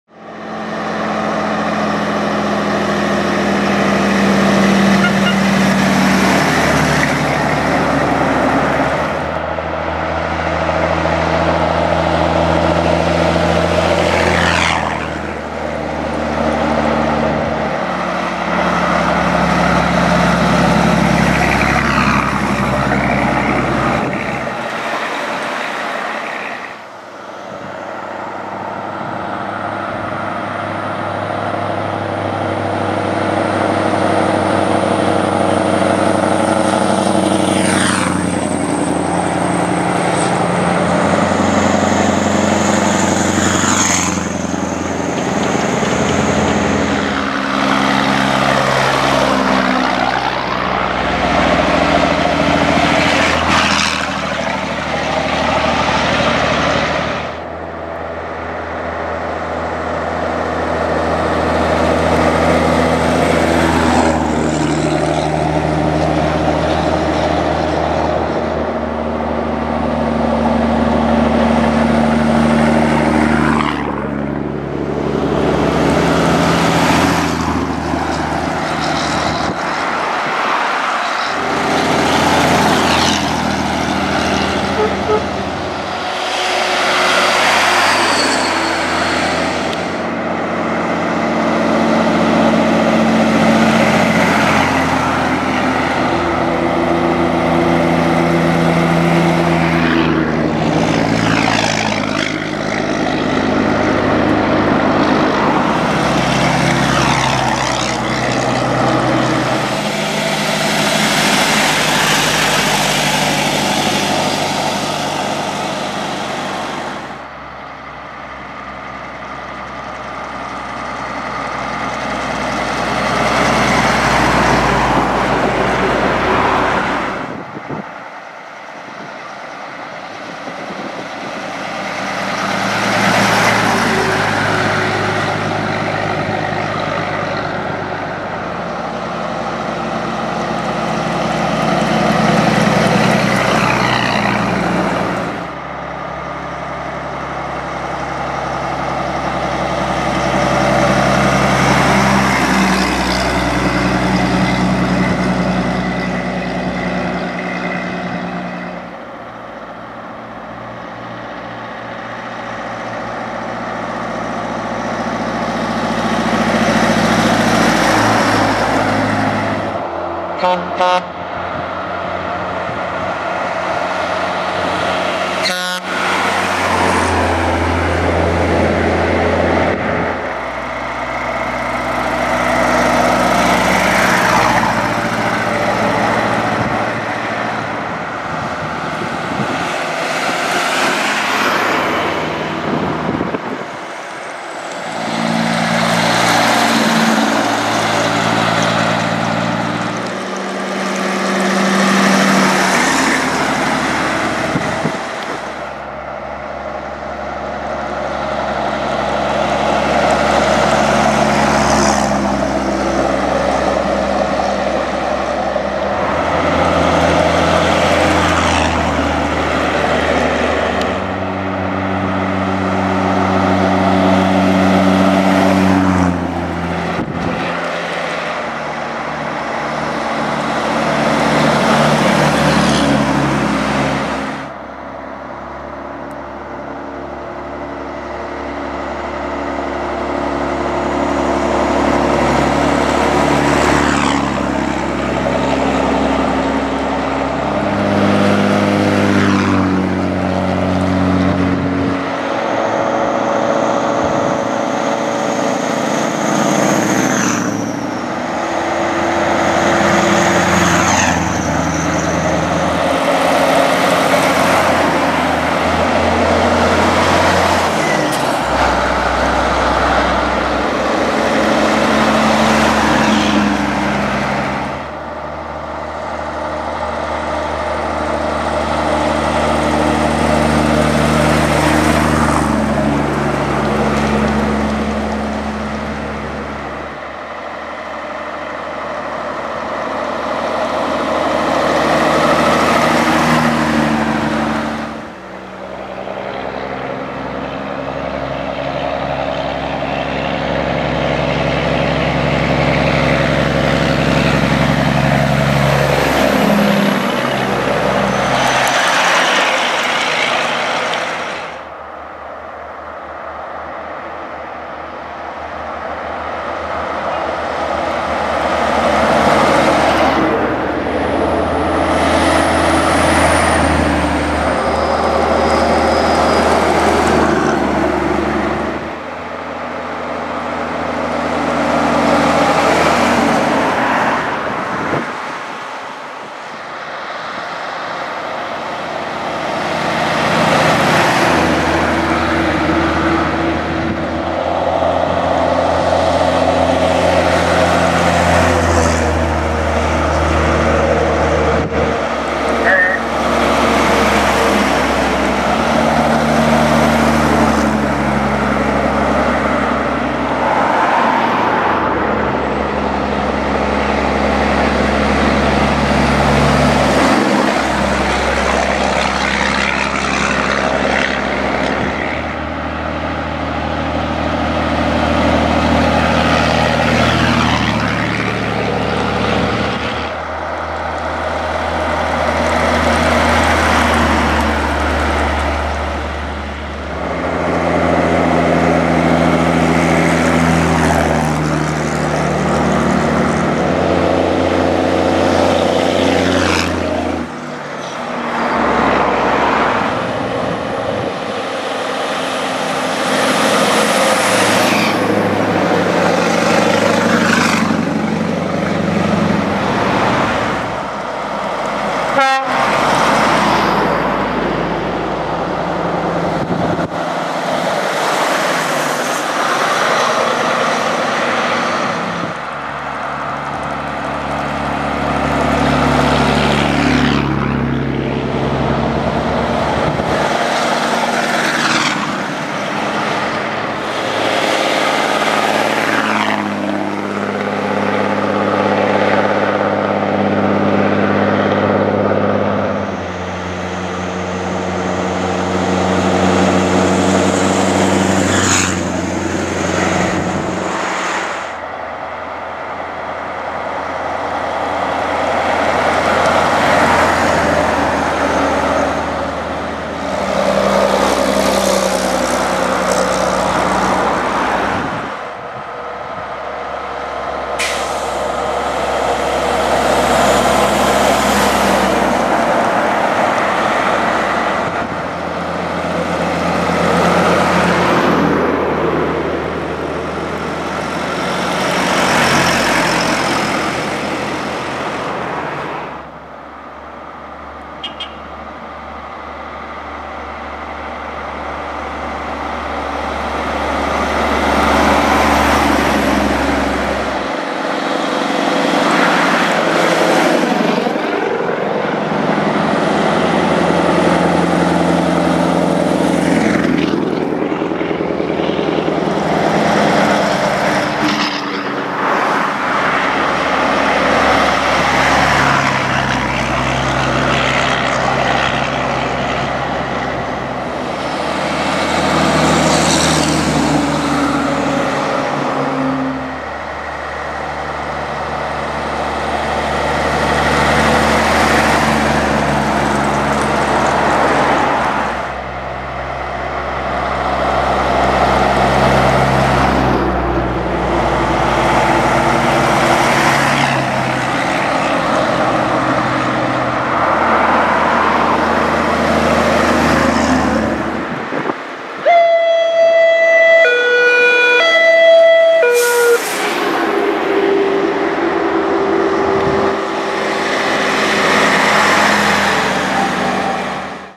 Åkarsvängen 2019 - Tunga Rallyt
Veteran Scania, Volvo, Peterbilt and other trucks going for a saturday trip around small swedish roads. Many of them sounds and looks really good too!